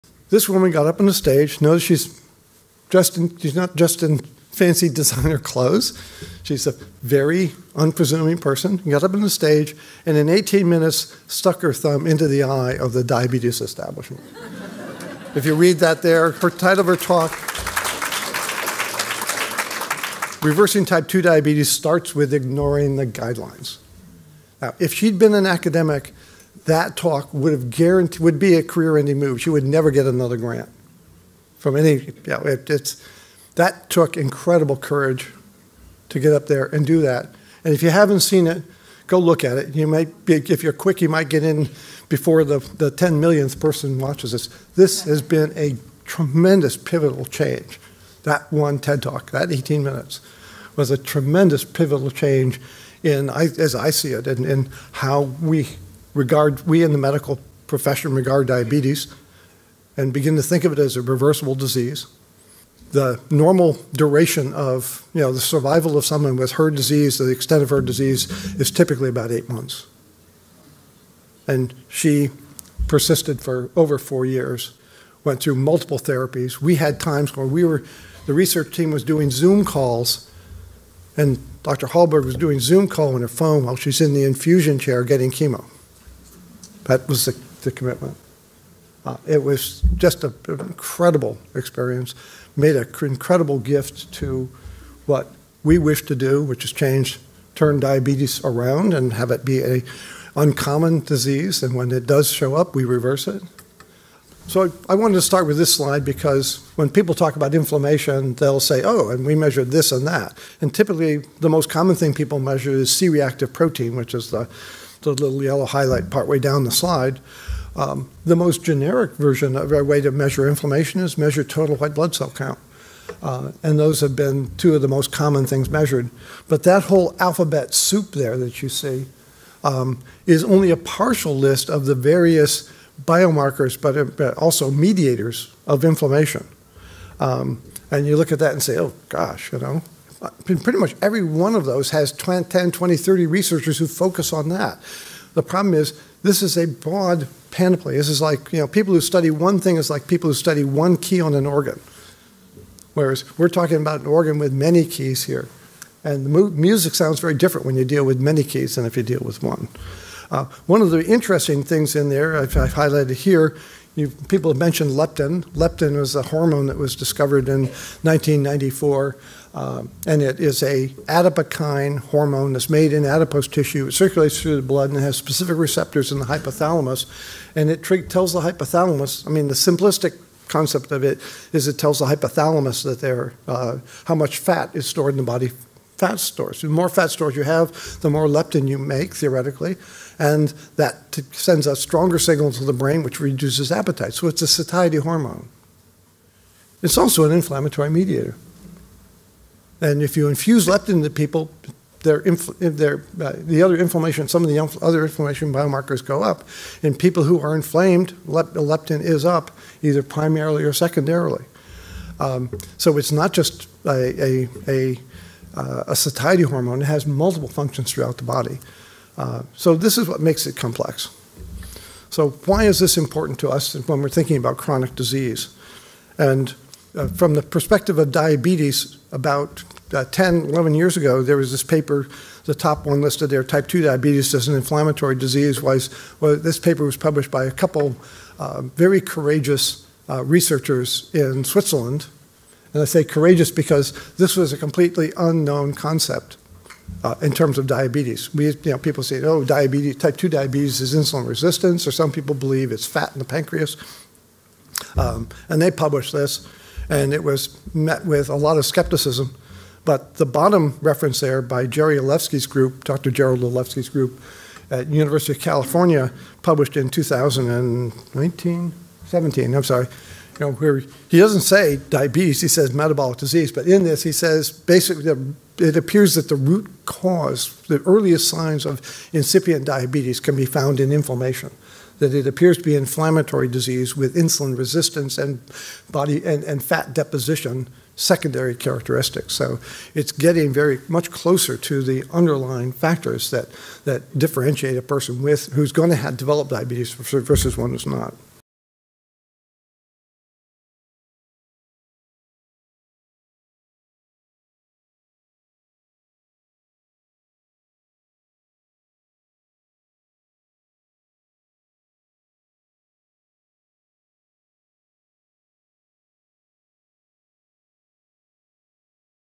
STEPHEN PHINNEY a | Type 2 Diabetes is an INFLAMMATORY DISEASE
presents episode 1358 | Dr Stephen Phinney

Dr Phinney pays tribute to the late Dr Sarah Hallberg…
-gave TED Talk on reversing T2D by Ignoring ADA Guidelines

[DoctorsToTrust.com, search on Sarah Hallberg
 -link is in video description below]

"That TED Talk was pivotal in changing
 the way medical community looks at T2D”

Dr Hallberg diagnosed with cancer…
-normal duration for that type is only 8 months, but she did
 every therapy available and kept working on our project
 for 4 years
-contributed to our goal of having diabetes become an
 uncommon disease and reversible

Most common measure of chronic inflammation is…
-CRP C-Reactive Protein, or
-total white blood cell count
-and many, many other measures
-but most researchers focus only on one measure

Leptin, adipokine hormone discovered in 1994…
-receptors in hypothalamus
-tells brain how much fat stores are in body
 -the more leptin, the less appetite
-an inflammatory mediator, when leptin up, inflammation is up

Paper published about 10 years ago:
-T2D is inflammatory disease
-totally new concept, met with lots of skepticism
Second paper, from UC California, 2017
-Earliest cause of diabetes is inflammation
 -with insulin resistance &
  fat deposition as secondary
  characteristics

Main purpose of this channel is to provide short, annotated key point videos
FOR YOU TO SELECT AND SHARE WITH LOVED ONES IN NEED.